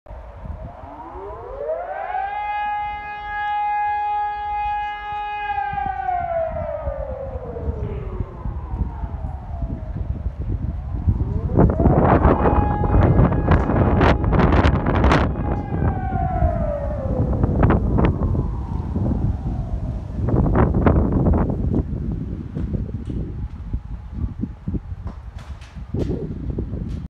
Ćwiczenia "Renegade" - 06.10.2020 - Gdynia, Chylonia - syreny alarmowe
PL # Ćwiczenia "Renegade" trwające od 6 do 9 października 2020 ćwiczenia wojskowe z wykorzystaniem systemów alarmowania ludności. Odbywają się one w celu domniemanego zagrożenia ze statku lotniczego klasy RENEGADE. Taki statek powietrzny może bardzo zagrażać życiu i zdrowiu ludzi.

EN # "Renegade" manevres are manevres with people's warning system (from 6th to 9th October 2020). These manevres are in order to danger through the air with airship "Renegade". This airship may pose a threat to people's life and health.